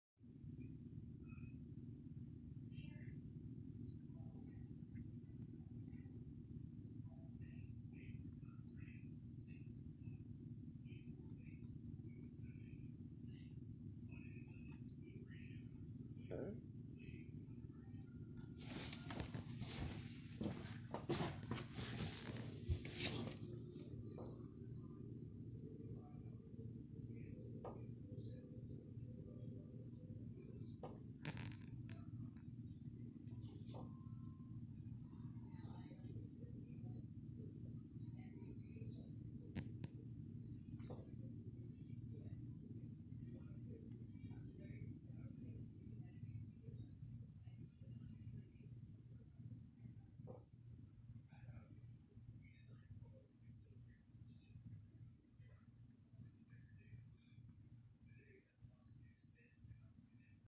A couple pseudomugil in a live food tank
It looks like this is how I seed my sponge filters now